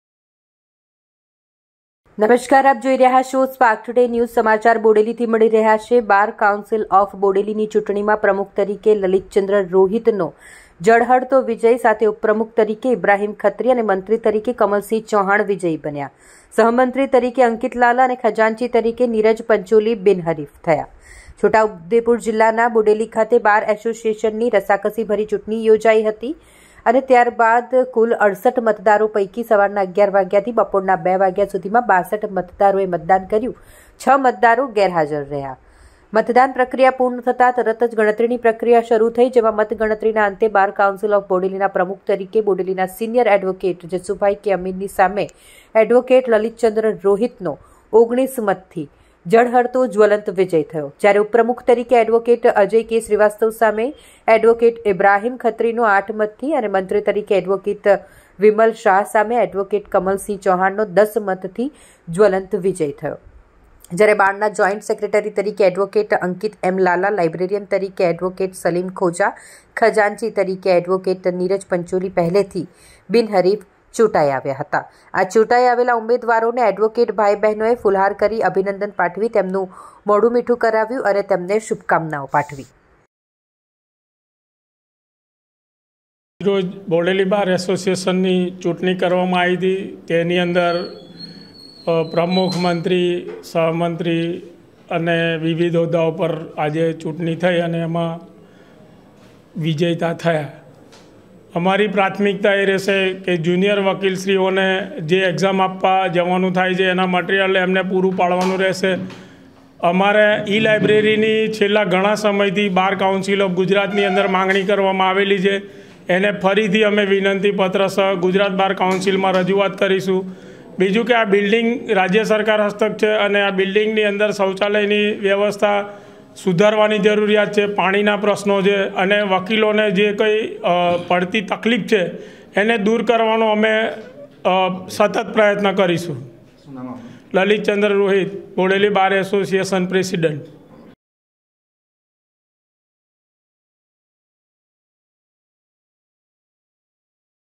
બોડેલી :બાર કાઉન્સિલ ઓફ બોડેલીની ચૂંટણીમાં પ્રમુખ તરીકે લલીતચંદ્ર રોહીતનો વિજય2022 |Spark Today News
બોડેલી : બાર કાઉન્સિલ ઓફ બોડેલીની ચૂંટણીમાં પ્રમુખ તરીકે લલીતચંદ્ર રોહીતનો વિજય